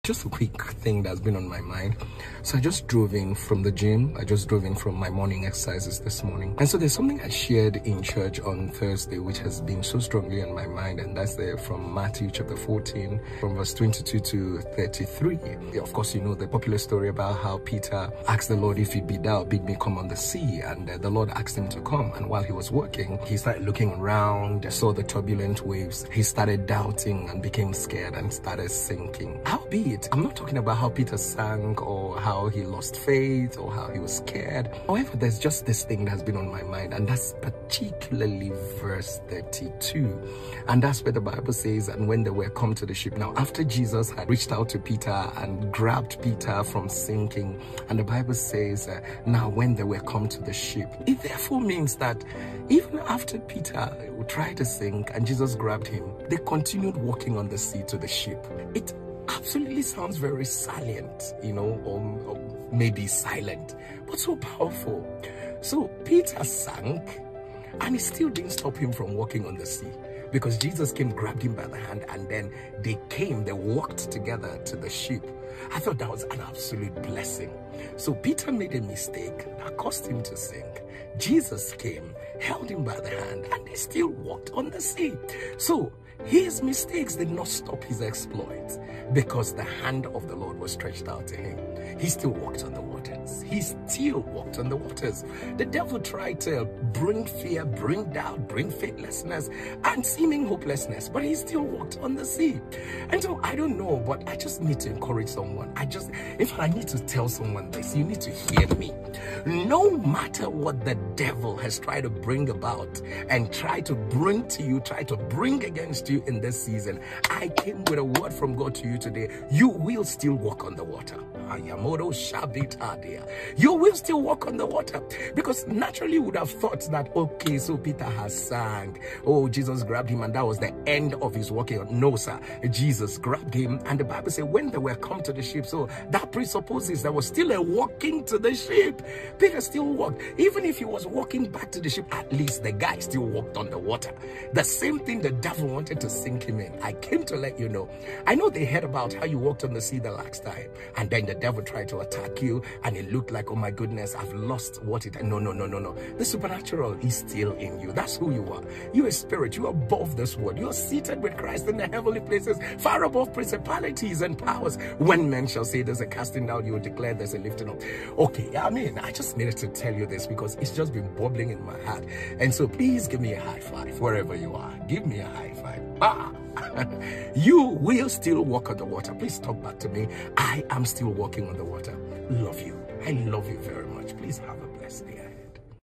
You will still walk on the water no matter what the devil brings your way | Pastor Jerry Eze
No matter what the devil tries to bring to you this season, you will still walk on the water. The supernatural is still in you- Pastor Jerry Eze